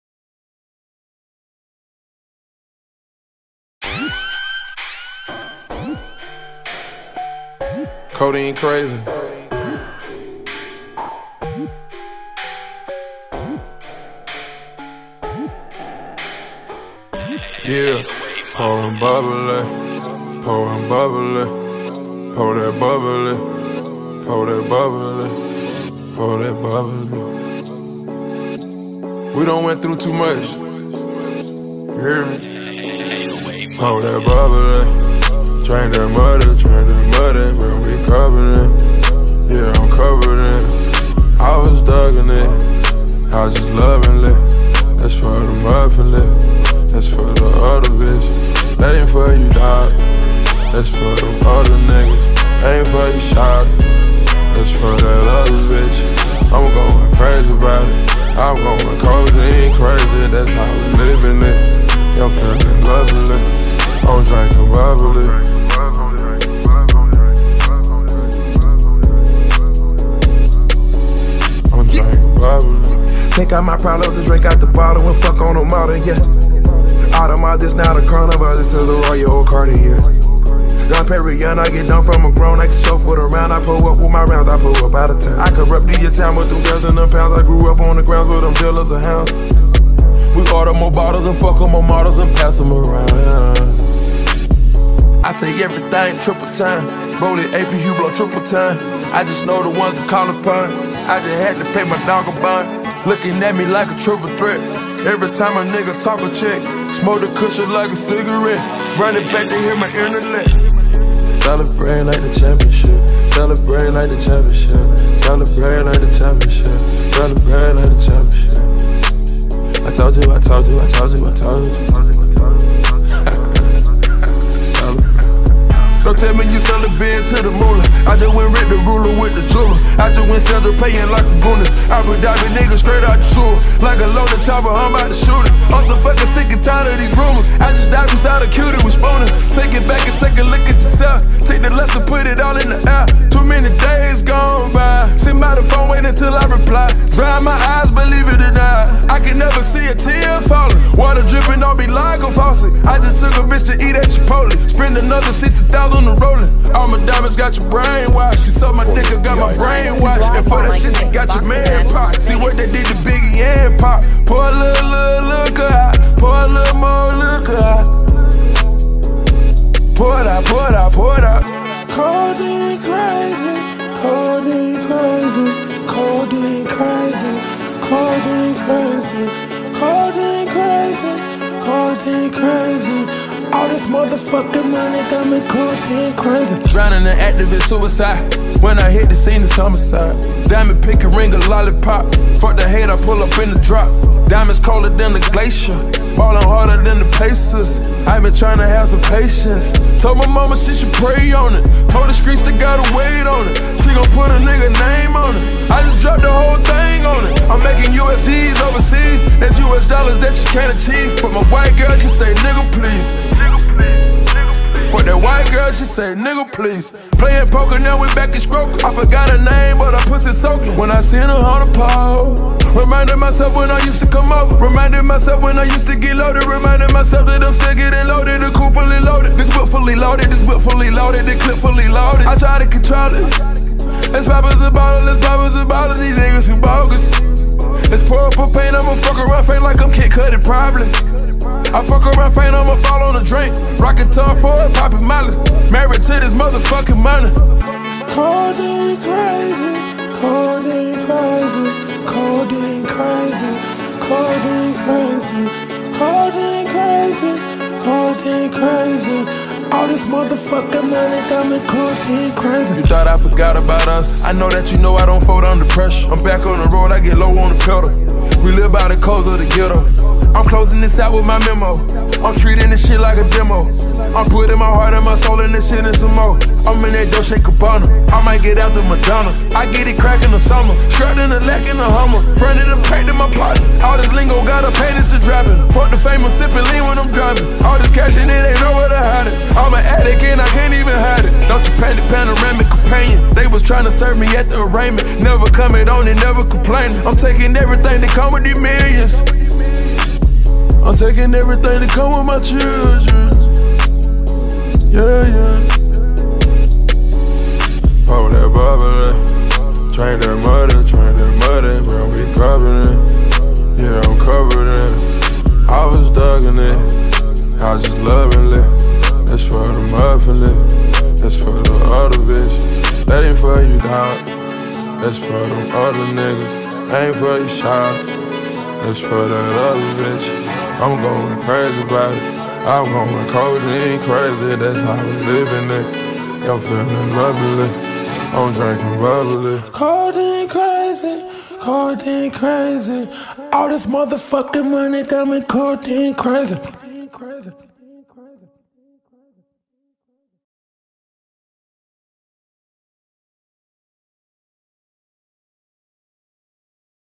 Griffin Shillingford - Kelvin High Clippers WHSFL Highlights
Griffin Shillingford - WHSFL Highlights
Kelvin Clippers - Winnipeg High School Football League

Not For Profit - Non-Monetized